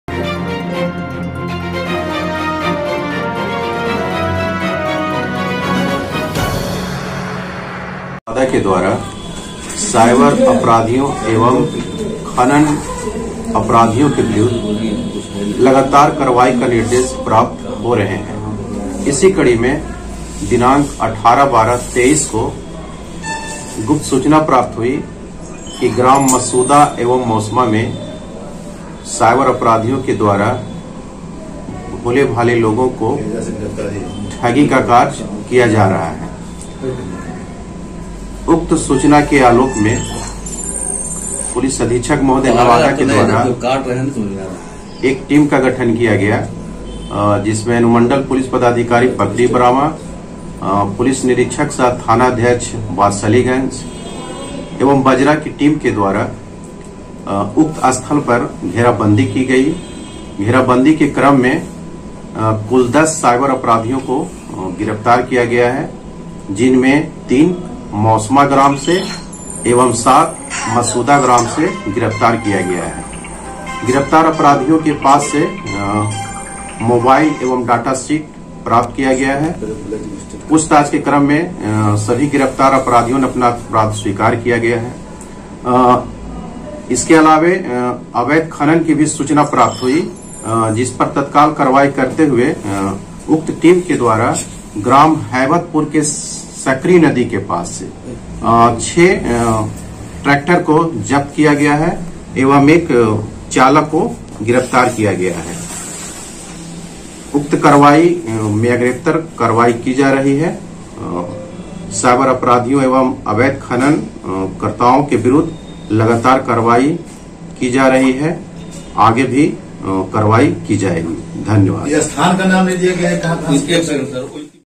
बालू माफियाओं पर पुलिस का सर्जिकल स्ट्राइक
बालू माफियाओं पर पुलिस का सर्जिकल स्ट्राइक ,कई अपराधी चढ़े पुलिस के हत्थे